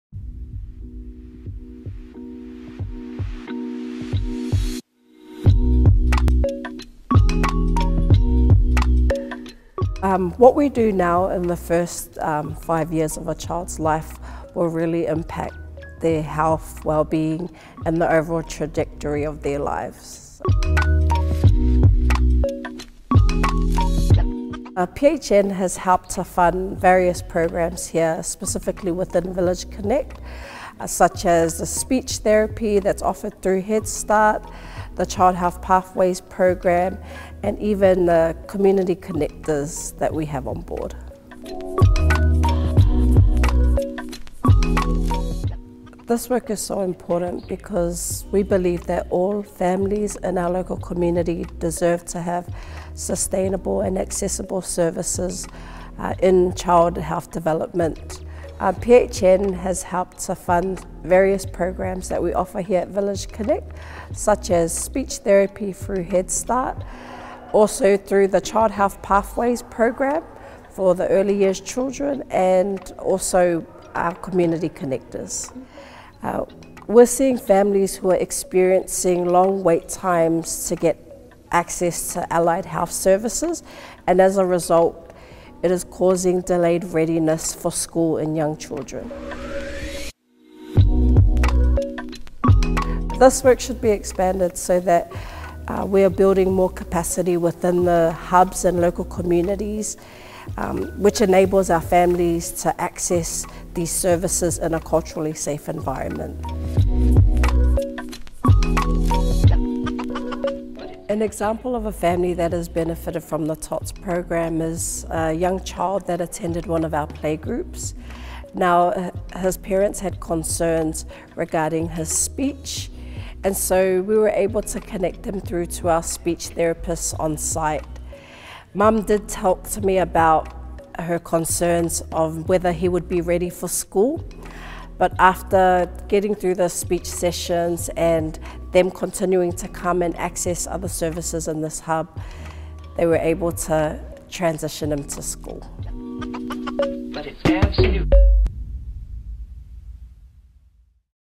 Supporting the First 2000 Days of a child's life with Village Connect
Brisbane South PHN partners to build an innovative early childhood health and wellbeing system that supports children's development over their 'First 2000 Days,' or their first 5 years of life.

Our Thriving and on Track (TOTs) program is one of our successful First 2000 Days strategies. It increases access to developmental screening and early intervention services for children aged 2.5 to 3.5 years in the Logan and Inala areas - with trials underway to expand across 0 to 5 years.

We spoke with Deborah Logoia, Community Support Coordinator, Village Connect, about the importance of expanding our Thriving and on Track program to build a stronger health system for children and their families.